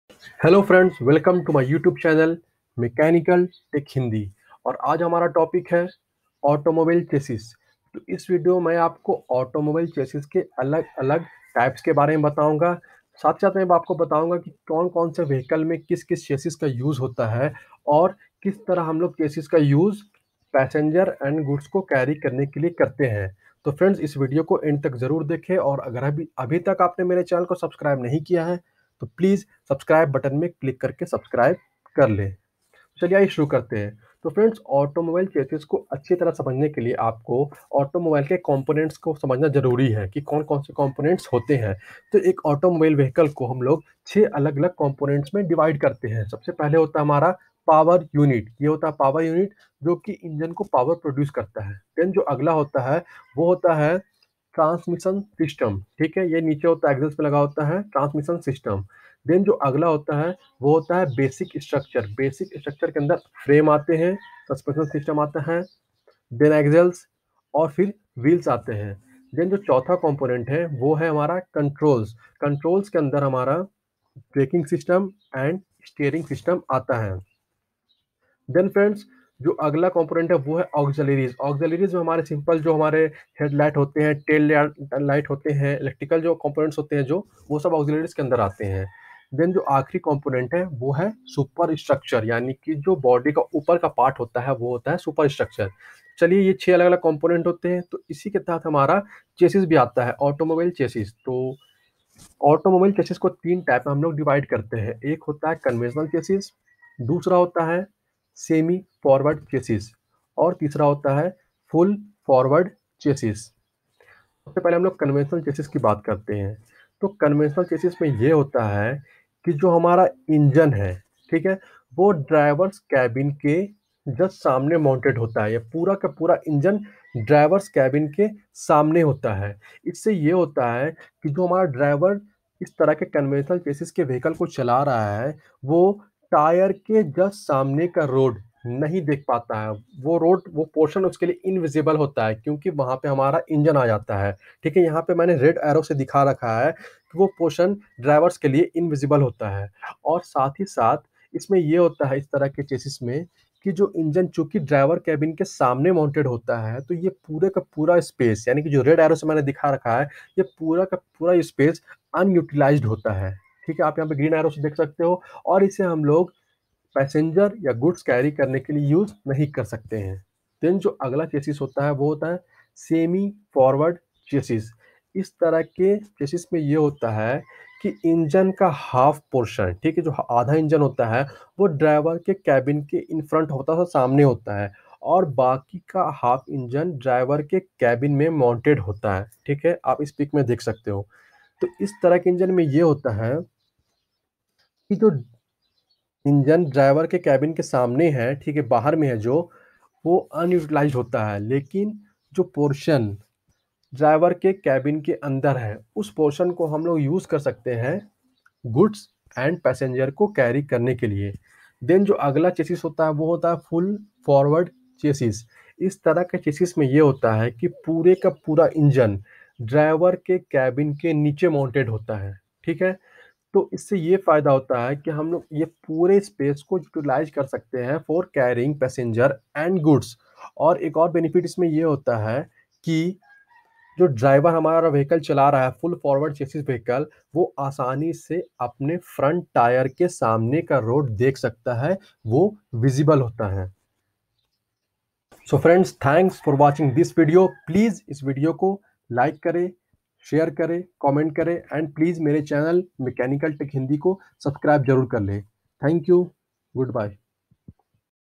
AUTOMOBILE CHASSIS & it's types
A chassis consists of an internal vehicle frame that supports an artificial object in its construction and use, can also provide protection for some internal parts.

1. Conventional chassis or frame-full chassis :
     In this type of chassis the body is made as a separate unit and then joined with ladder frame. It supports all the systems in a vehicle such as the Engine, Transmission system, Steering system, Suspension system.

2. Non conventional or frameless chassis :
    In this type of chassis the ladder frame is absent and the body itself act as the frame. It supports all the systems in a vehicle such as the Engine, Transmission system, Steering system, Suspension system.